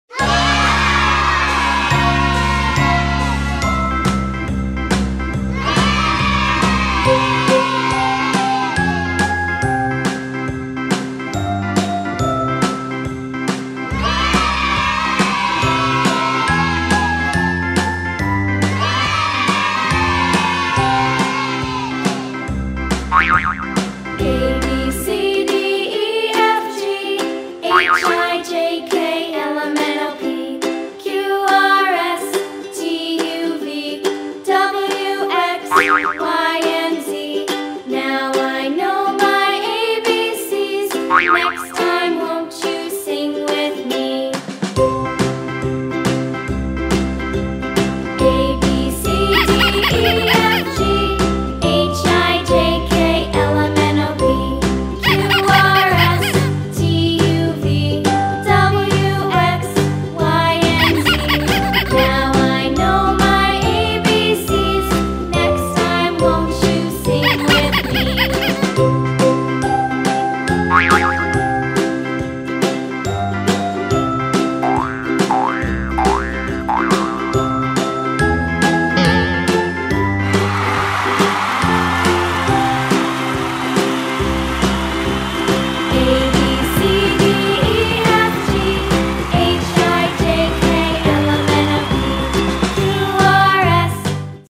Tayo The Little Bus Toys, Learn Colors with Bus Toys and Finger Paint
Learn colors video with Tayo The Little Bus  toys for kids. Bus toys are getting covered in finger paint and then the finger paint is coming off the vehicles. Then the bus toys drive over the paint to make a big mess. Last the buses drive backwards in reverse over the paint to take the paint off. Children who like Tayo the Little Bus toys will love this video.